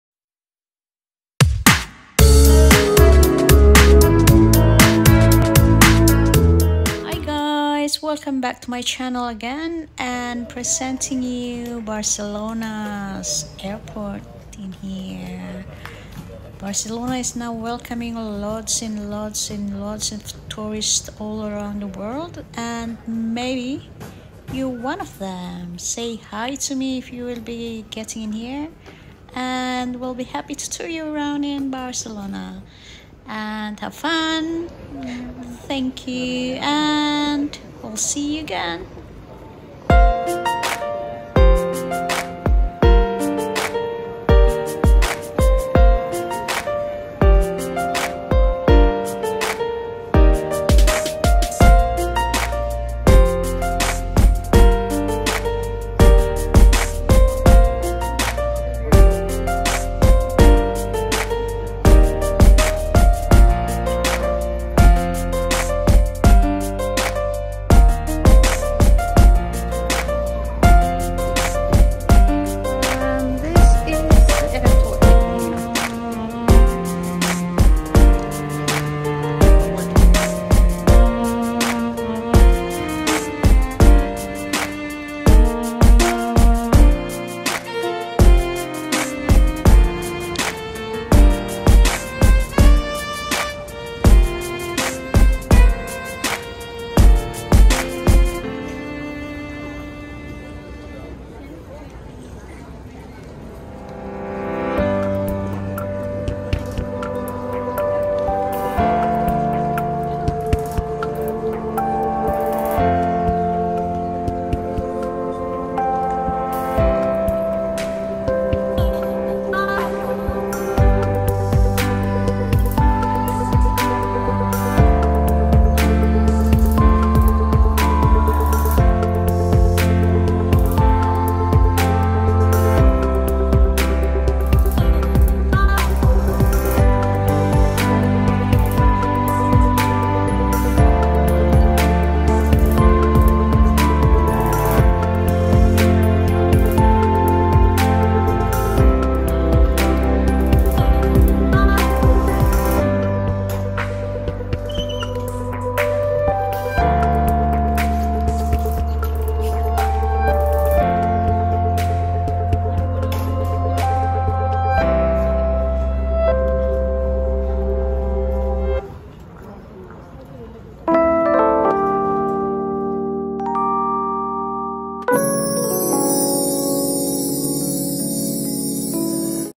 Barcelona El Prat Airport#spain#airport#travel#tourists
Barcelona International Airport (also known as El Prat de Llobregat Aeropuerto or Aeroport de Barcelona) is situated 13 km South West of the city centre. The airport abbreviation is (BCN). This abbreviation is often referred to by online flight reservations services and will be most likely printed on your airline ticket.